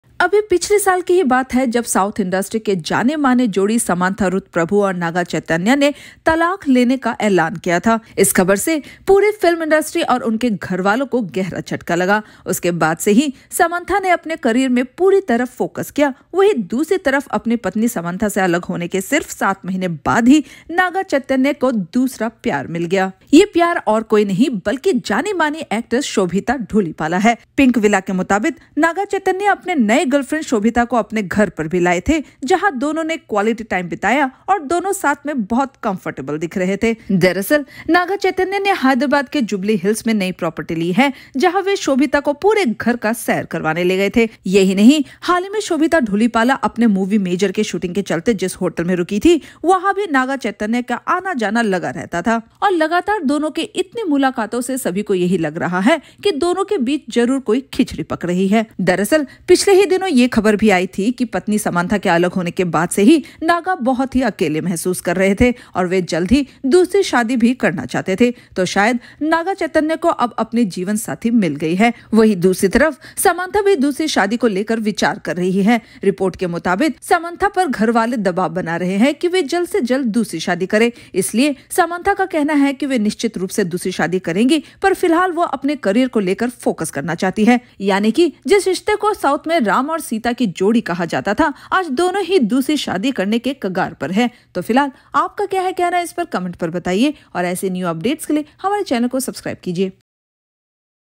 EXCLUSIVE: Naga Chaitanya Dating Major actress Sobhita Dhulipala after separation from Samantha
EXCLUSIVE: Naga Chaitanya dating Major actress Sobhita Dhulipala after separation from Samantha Ruth Prabhu
According to our source and an eyewitness, Naga Chaitanya was spotted with Made In Heaven fame Sobhita Dhulipala at his new abode.